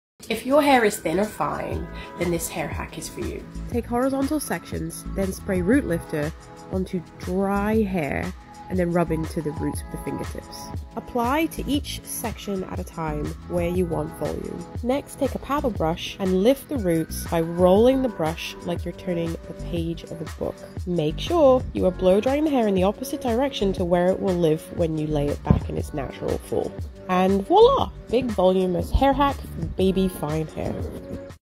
Try This Thin Hair Hack | Hair.com by L'Oreal
Did you see that foam action?! 😍 This hack from @Sarah Bramham is perfect for you fine-haired beauties.